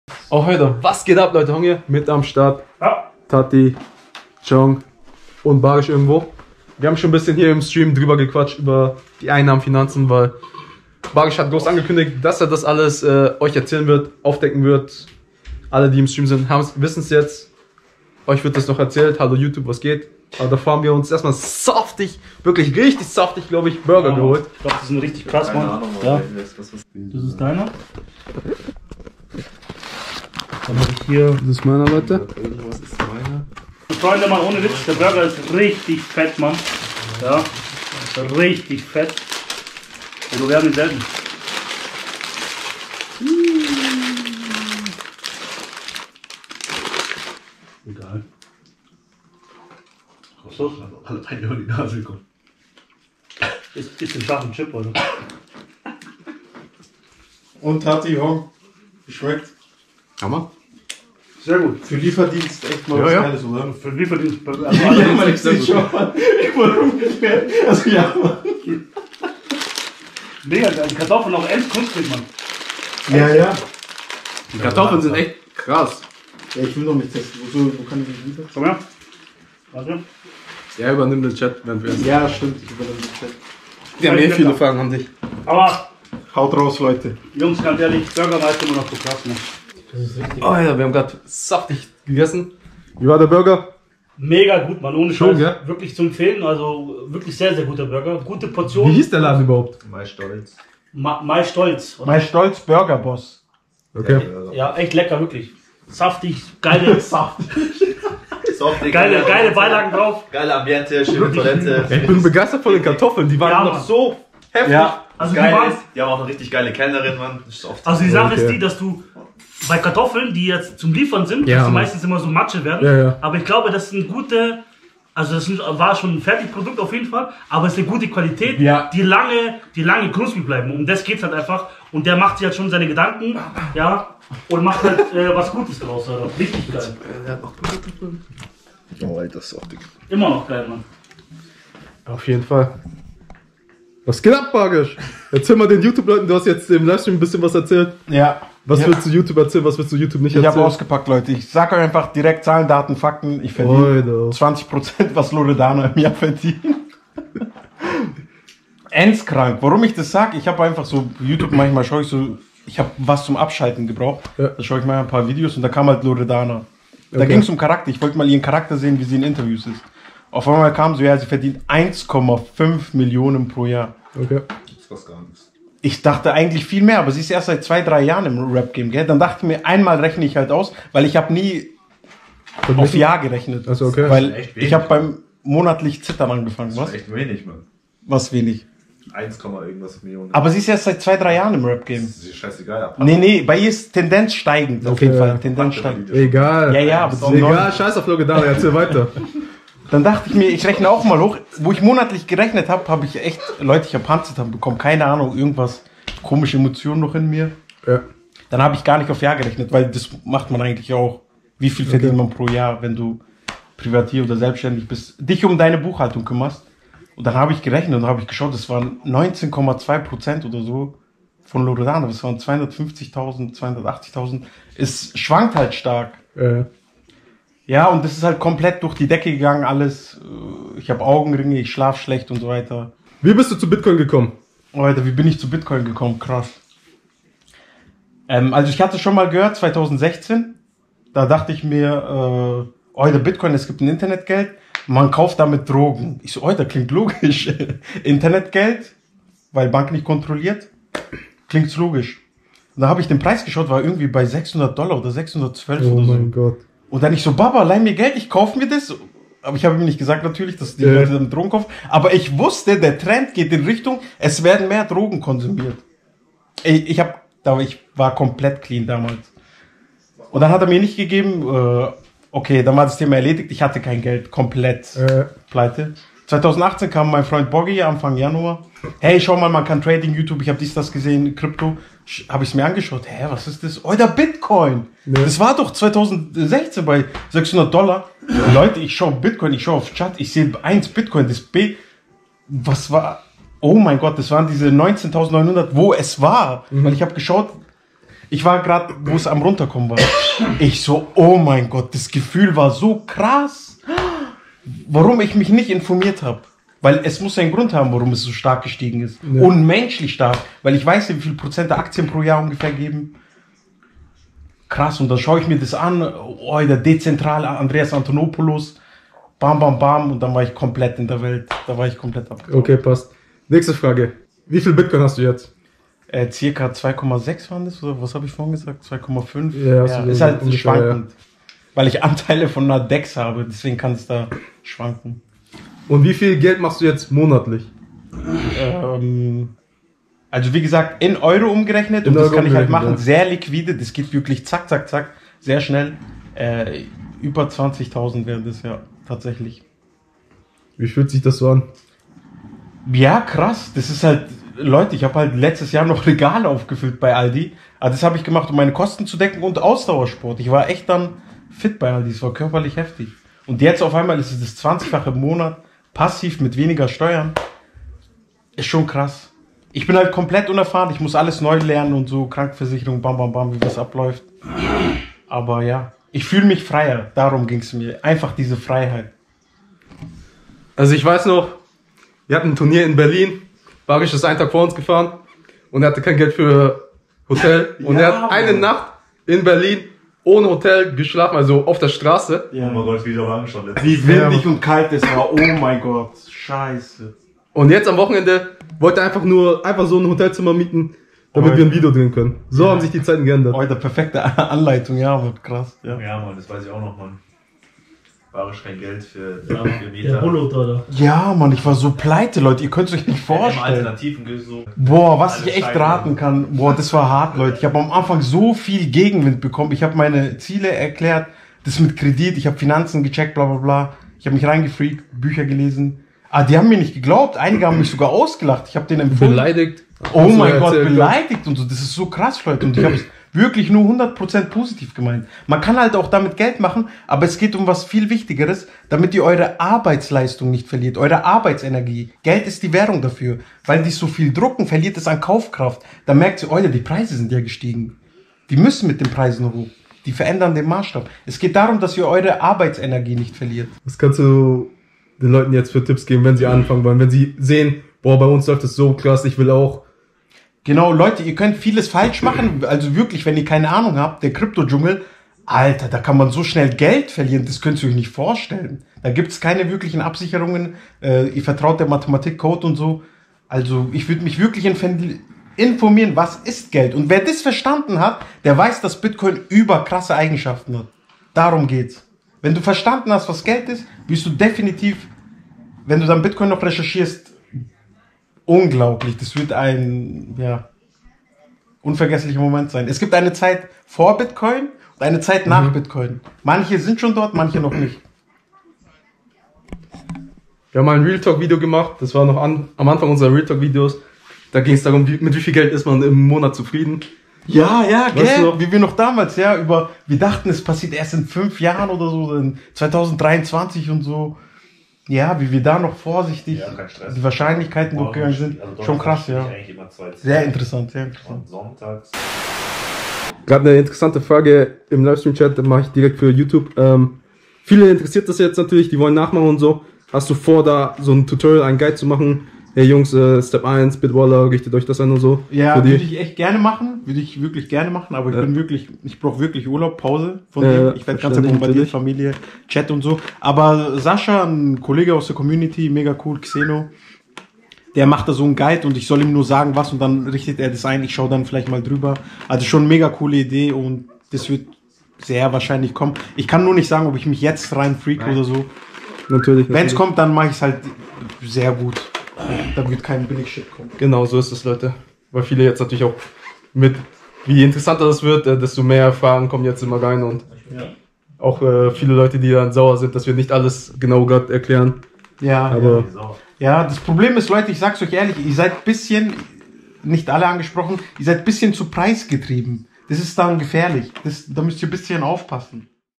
Er verrät seine BITCOIN EINNAHMEN 😱🤫
euda Baris verrät euch seine monatlichen Einnahmen durch Crypto
er hat seinen Job geschmissen und ist jetzt finanziell frei

🛍️ für alle die Sushi selber machen wollen*: